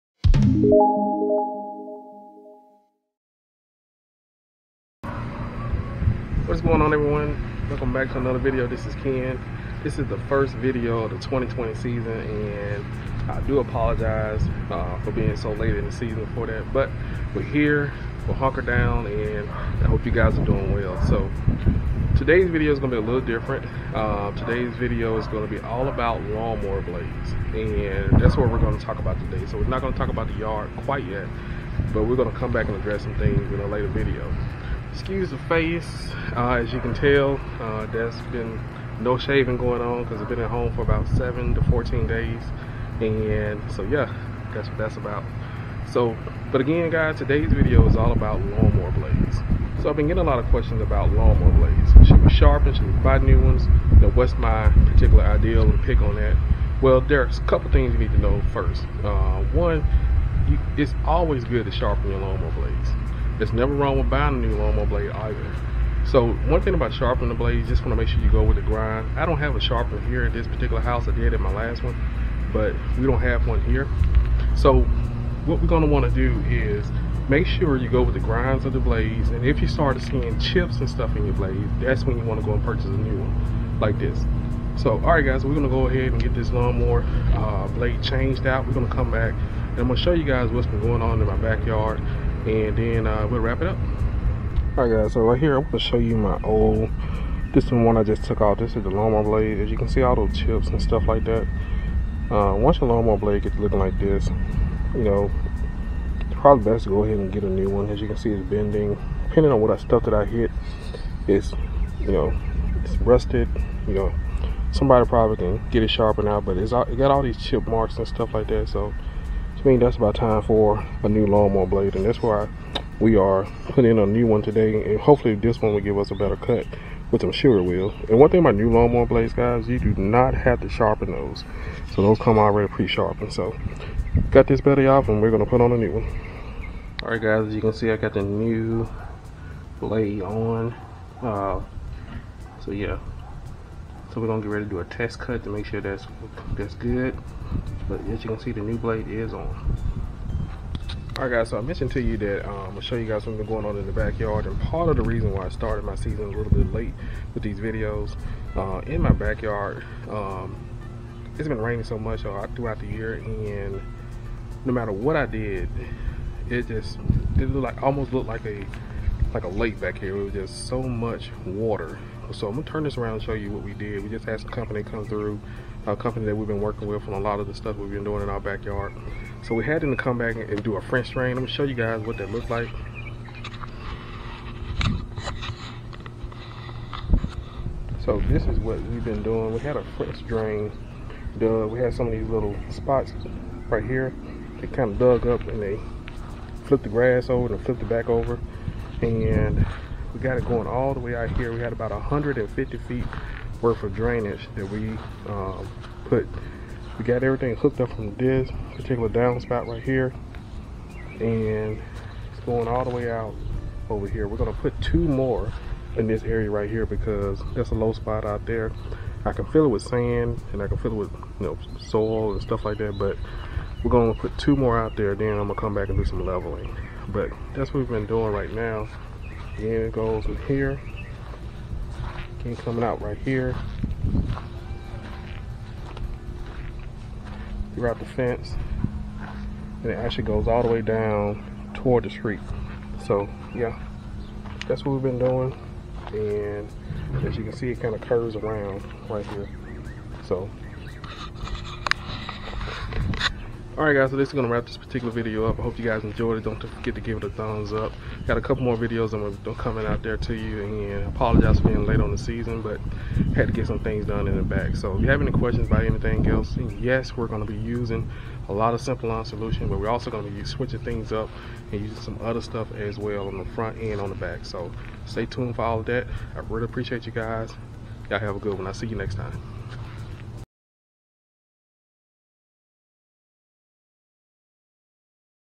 When should you change your lawn mower blade/ How to!
In this video, I discuess why you should change your blades to your lawn mower. Also when and if you should buy new blades for your lawn mower.